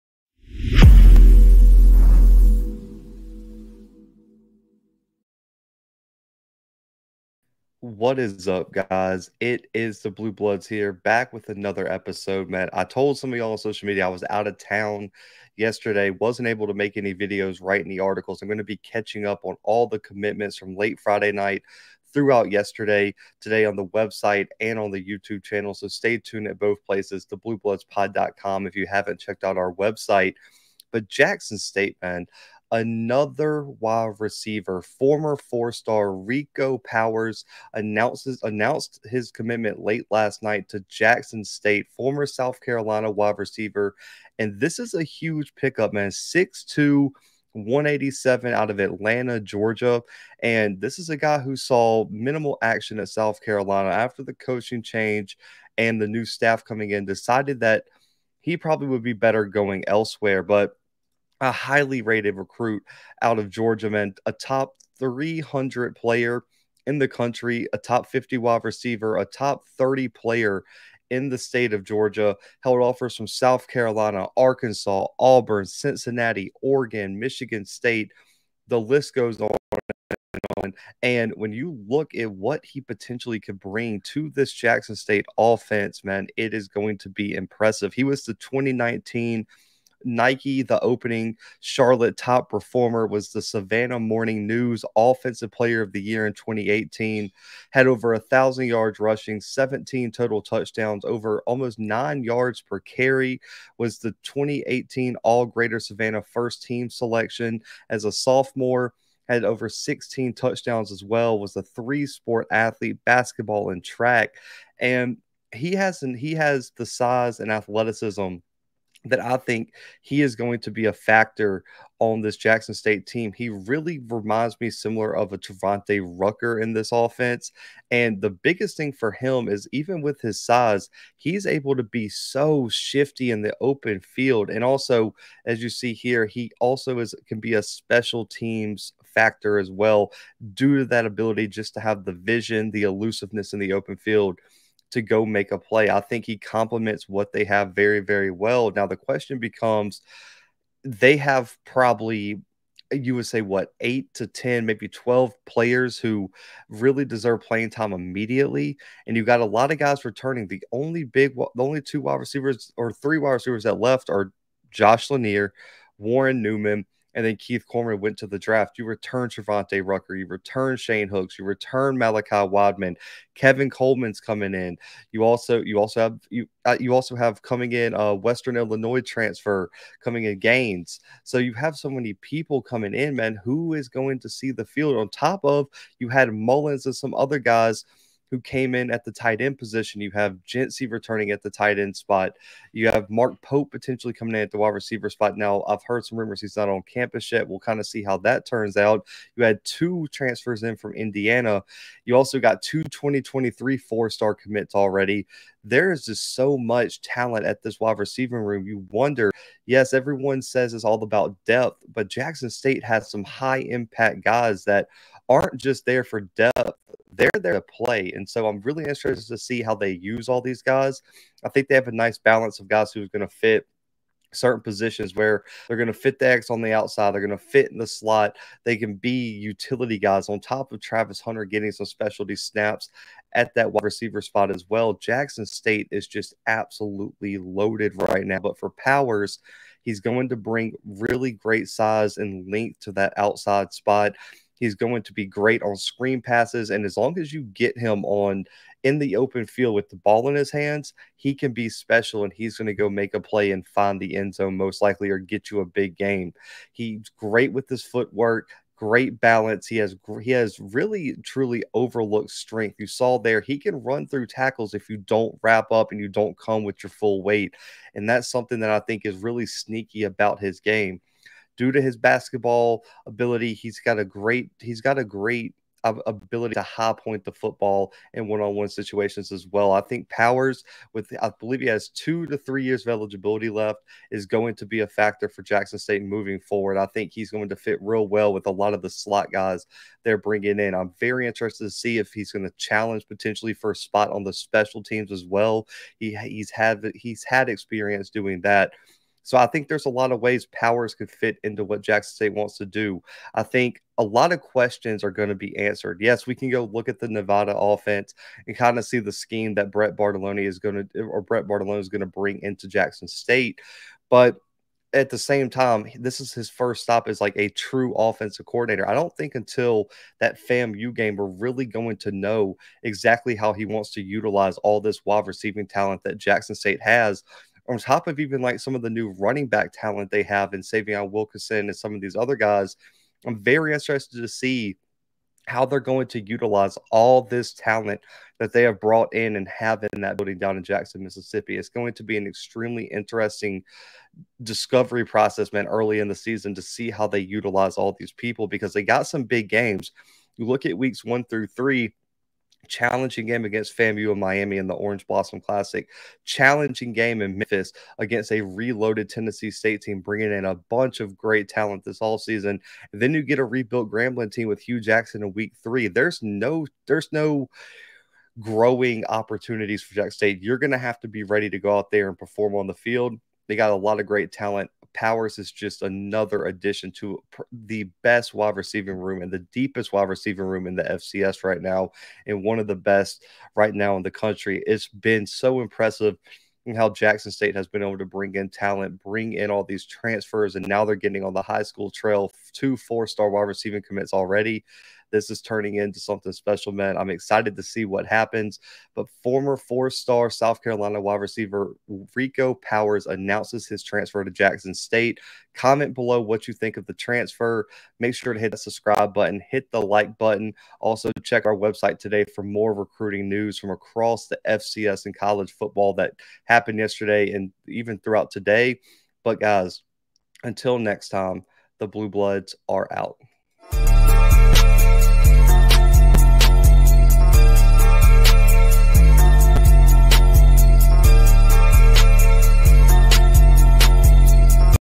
Jackson State Lands Former 4-Star South Carolina WR Rico Powers | The Bluebloods
On this episode of The Bluebloods, Zach McKinnell breaks down the recent announcement that former 4-star South Carolina WR Rico Powers has transferred to Jackson State. What will Powers bring to the Jackson State offense? How will Powers fit into the new Jackson State offensive scheme? Can Powers find his role in a loaded Jackson State WR room in 2022? All this & more right here on The Bluebloods!

The podcast is available on all podcast streaming platforms!